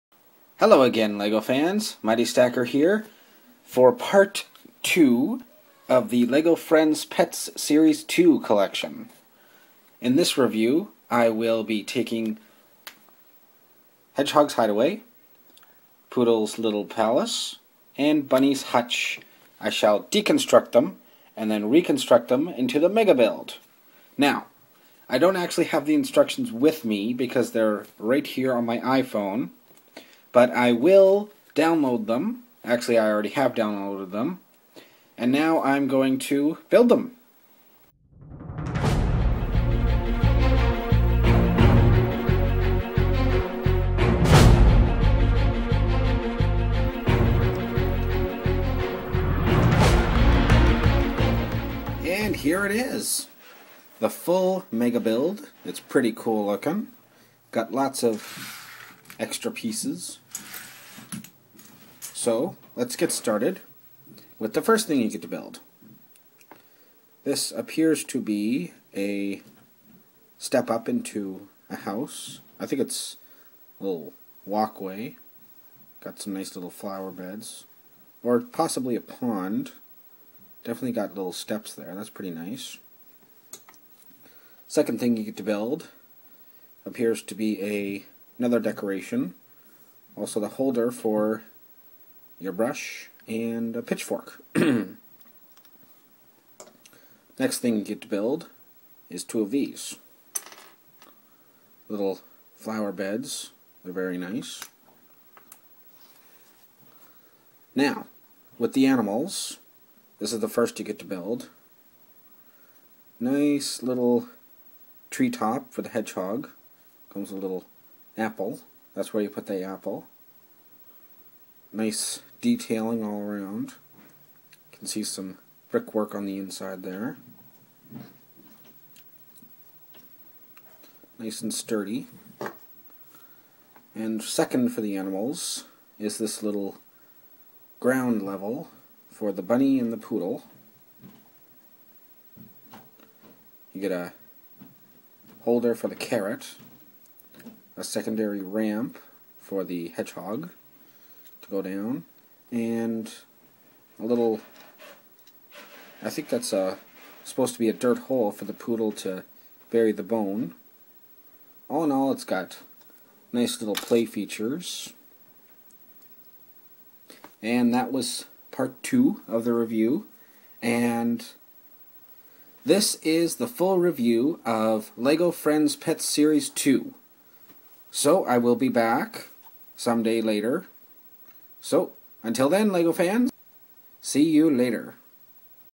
Lego Friends pets complete series 2 review, part 2: The Meg
Here's part 2 of my Lego Friends pets complete series 2 collection, the Megabuild.